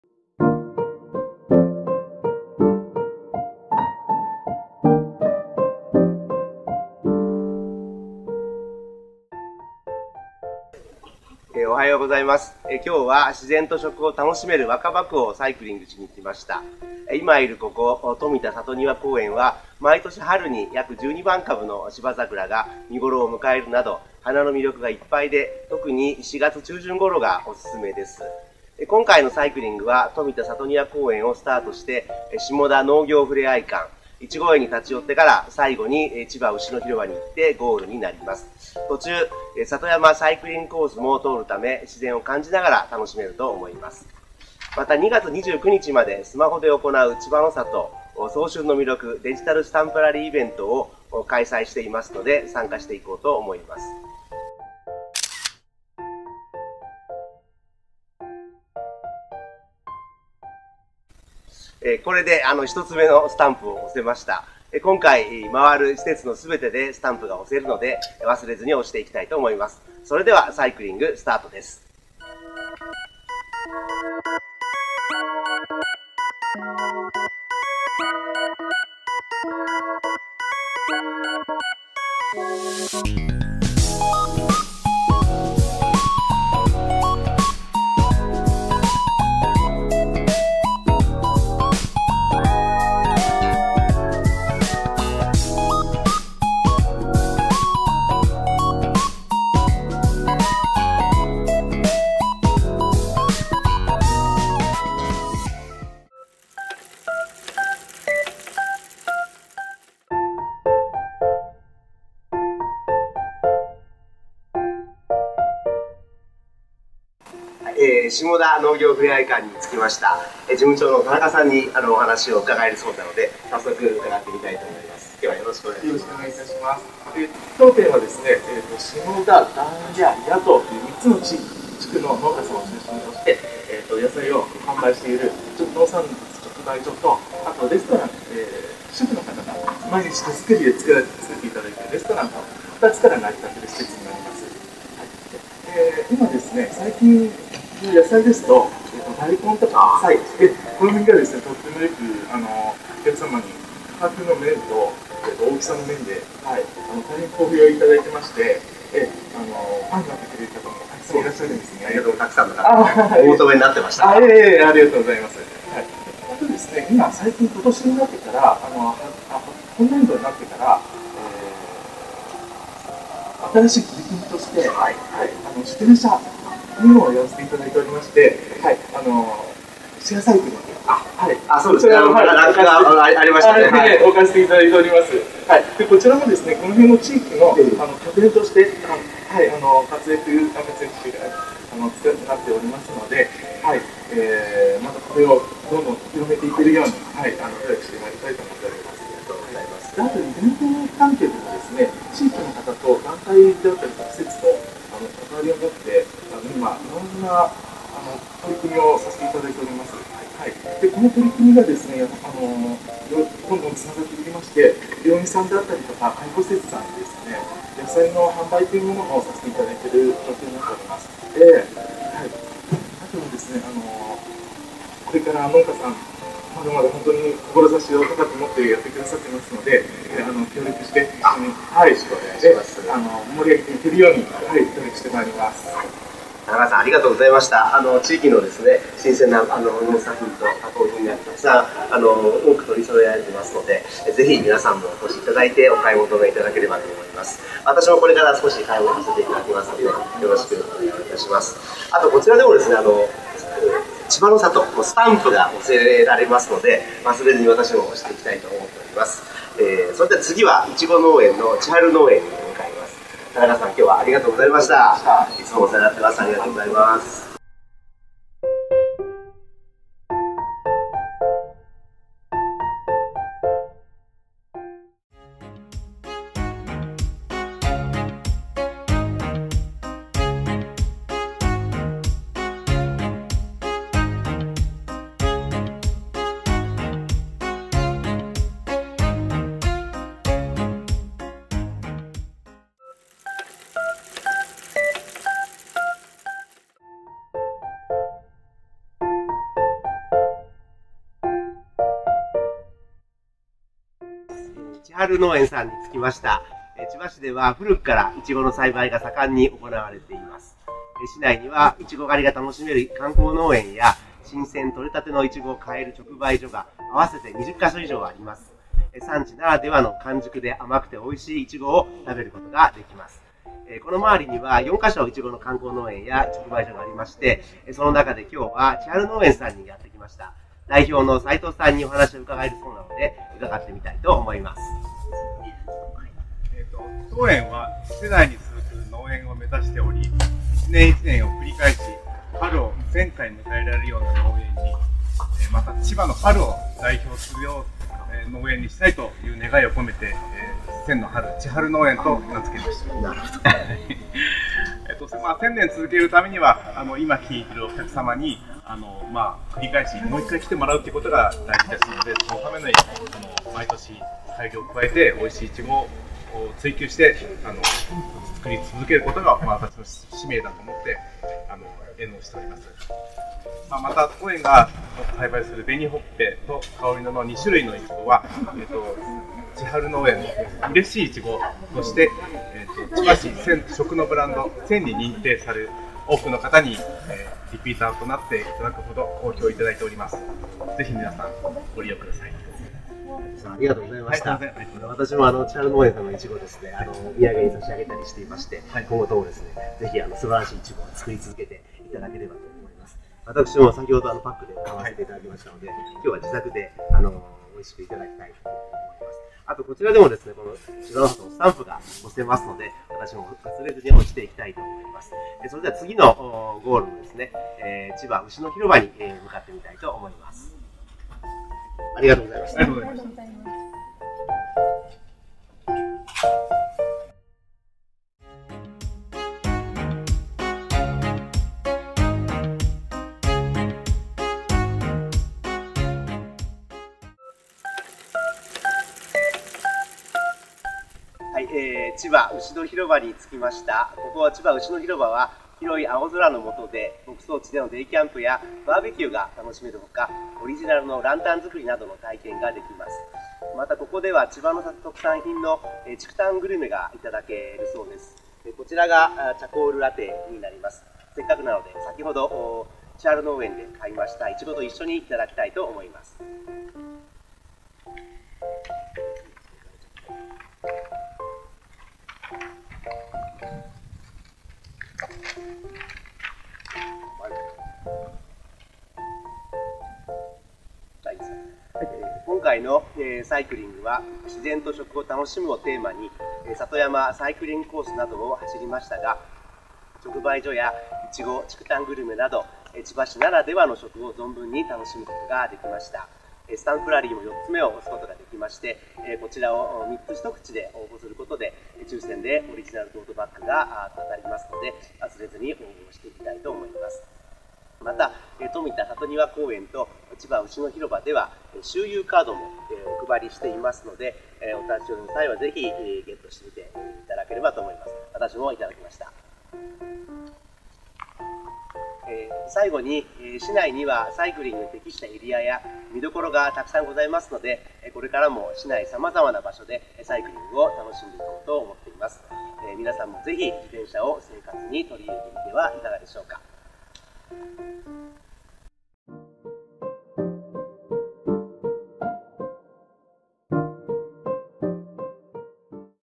【千葉市長サイクリング②】～若葉 自然と食 編～
千葉市らしい生活スタイルである『ちばチャリスタイル』の実現に向け、自転車を楽しく安全に、自発的に利用していただくため、様々な取組みを行っています。
今回はちばチャリスタイル普及のため、千葉市長が若葉区のまちをサイクリングしました。

↓今回立ち寄った施設のご紹介↓
富田さとにわ耕園

建設局道路部自転車政策課企画整備班